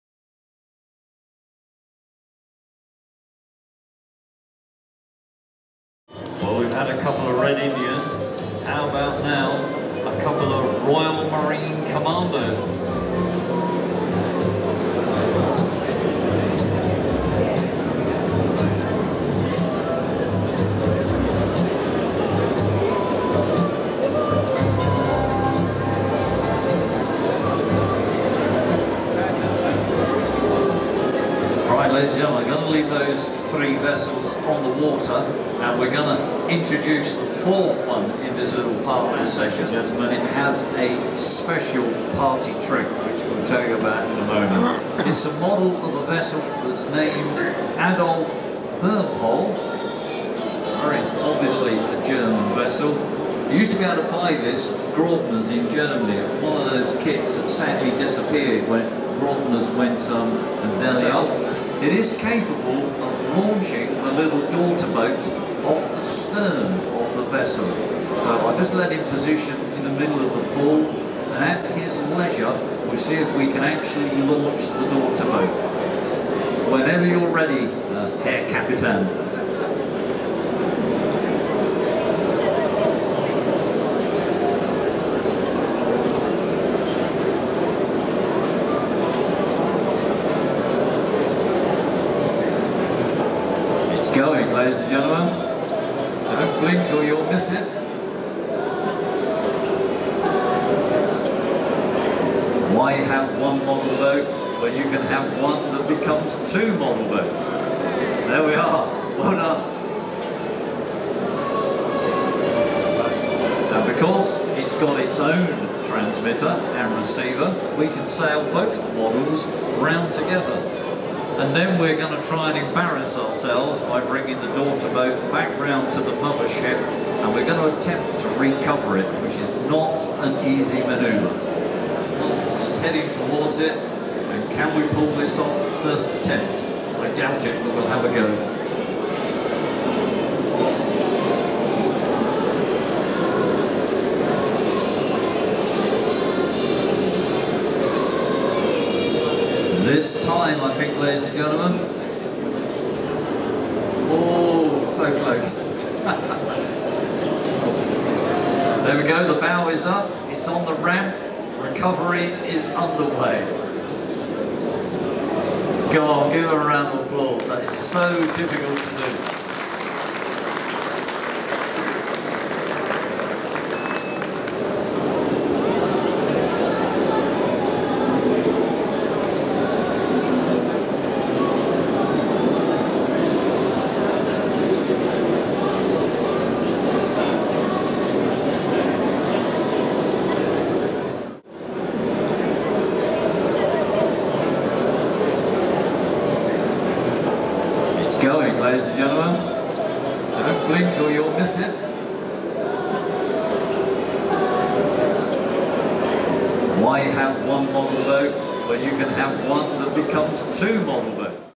RC Launch and Recovery LIFEBOAT and Rowing MARINES
Filmed at the International Model Boat Show. Some of the more curious watercraft on display over the weekend. Great fun to watch. Commentary provided by Steve Dean.

Filmed at the International Model Boat Show, hosted by Meridienne Exhibitions in Warwickshire, using the Panasonic AG-UX180 4K pro camcorder by 'Dom Mitchell' of Essential RC YouTube channel.